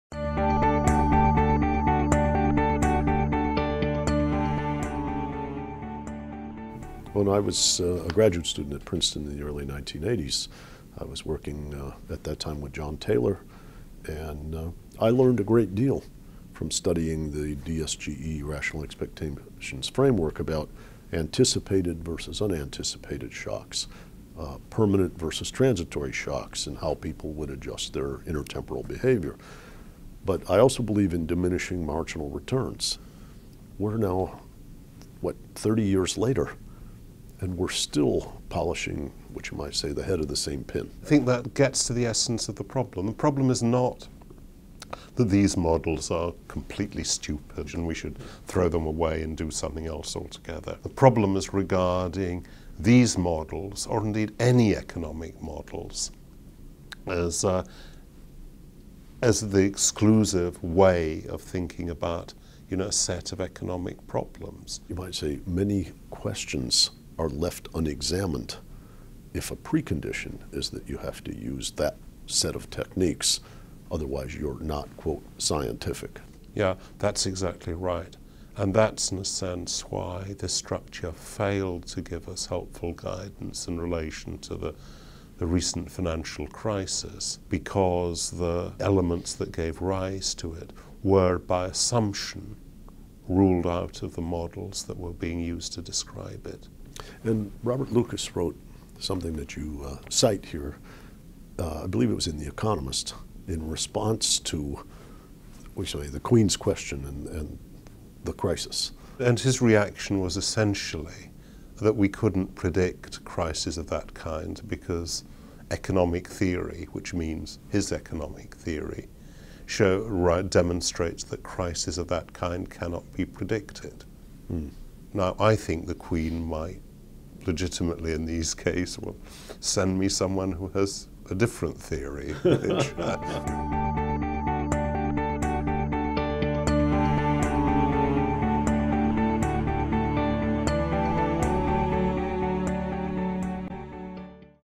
John Kay: Polishing the Head of the Same Pin (2/5)
In part two of this INET interview, John Kay discusses the inflexibility of current economic theory and how this has lead limited the economics profession. By insisting on certain simplifying assumptions to maintain the pretense of being scientific, economics has left unexamined many important questions that don't fit the pre-prescribed, rigid assumptions. This, Kay suggests, is part of why economics failed to predict or respond adequately to the financial crisis.